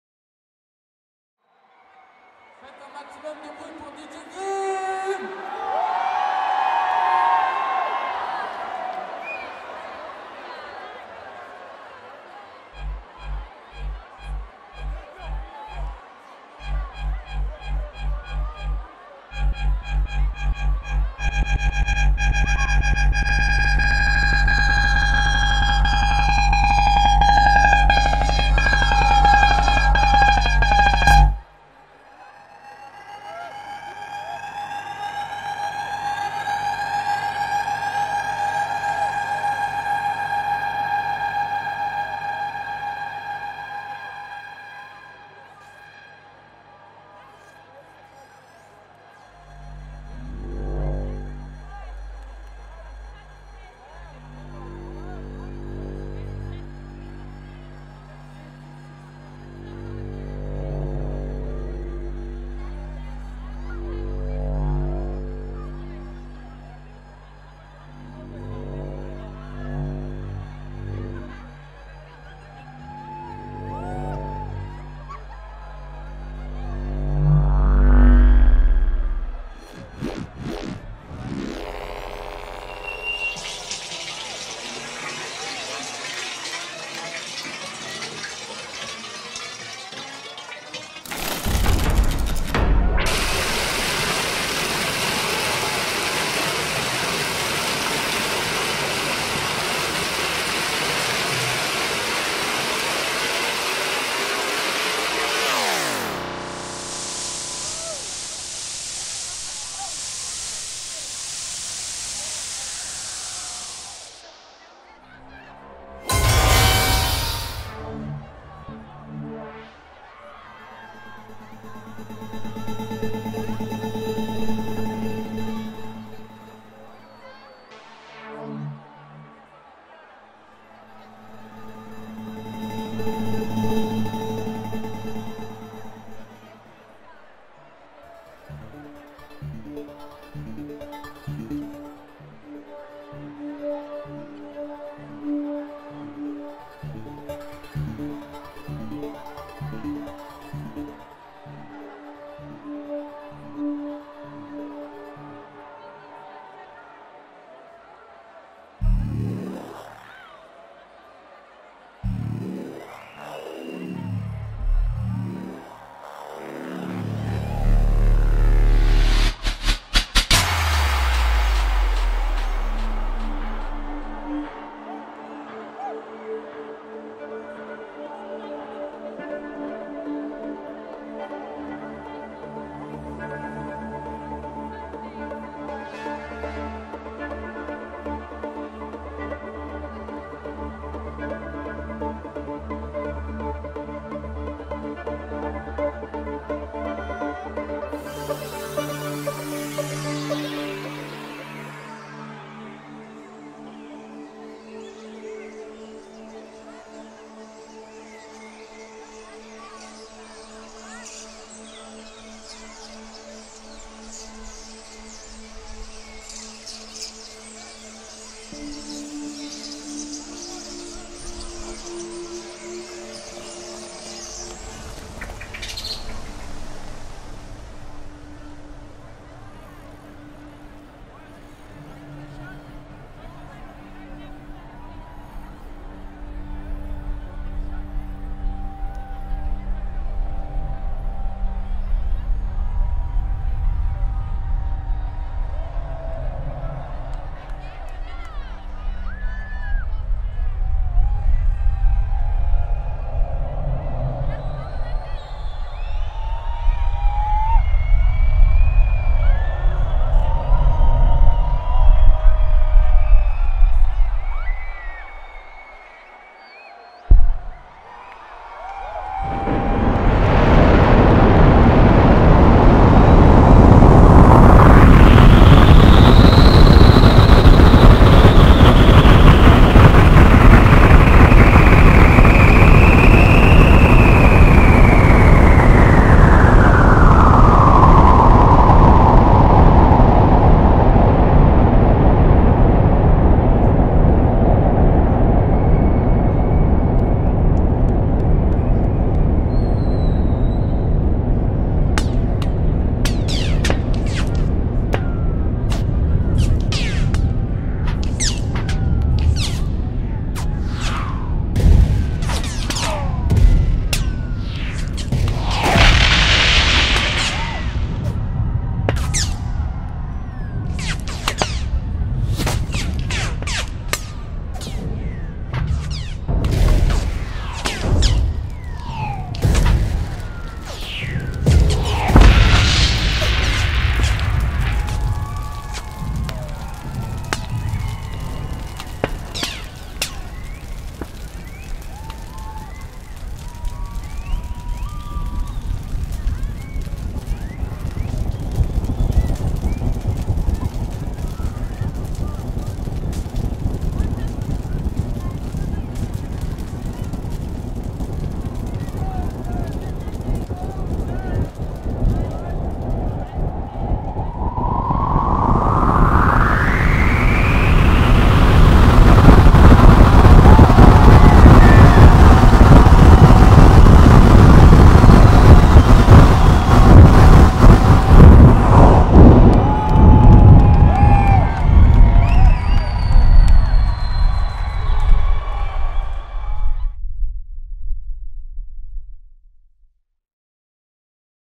VOLCANO by STUDIO CORIUM - part one
Giant setup and Mapping installation for The Uniparty 5  and "la fête du printemps 2014"

Stage design: Mika Ventura (Studio Corium)
Building Setup: Adrien Boulanger
Lighting & laser: MKLAZER & Alex
Image : Catherine Baroni (Studio Corium)
Equipment rental: Hyperson, Lumens8, Spectralight